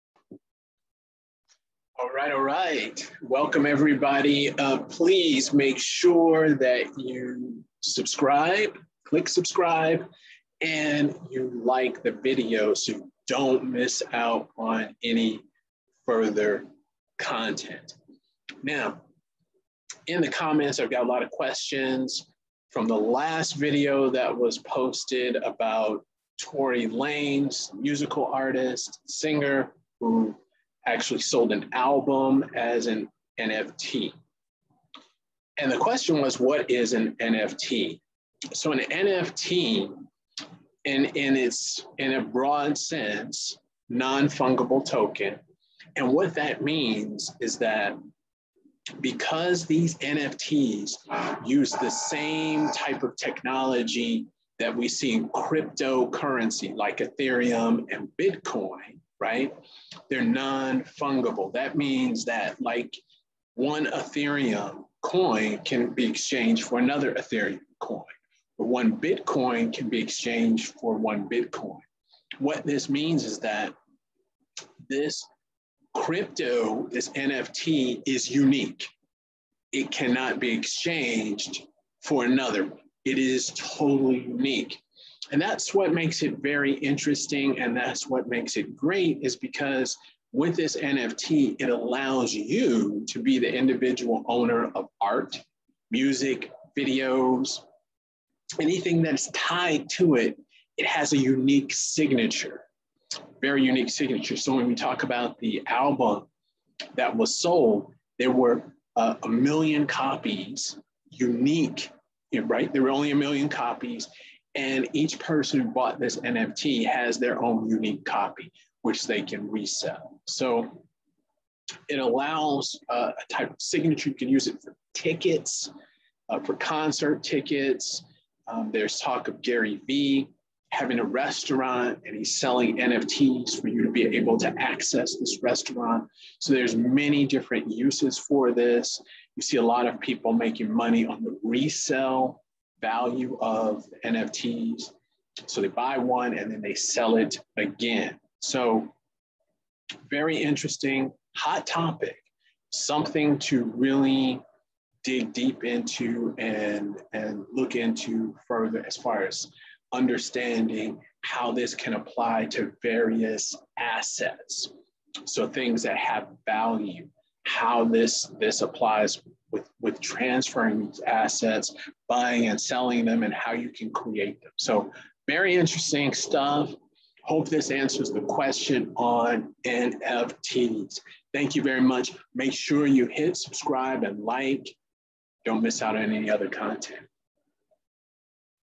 What You Need To Know About Non-Fungible Tokens (NFTs)
TORY LANEZ REPORTEDLY SELLS 1 MILLION COPIES OF $1 NFT ALBUM IN 57 SECONDS, PART 2. The follow-up video where Mfon Akpan explains NFT's. An NFT is a digital asset that represents real-world objects like art, music, in-game items, and videos. They are bought and sold online, frequently with cryptocurrency, and they are encoded with the same underlying software as many cryptos.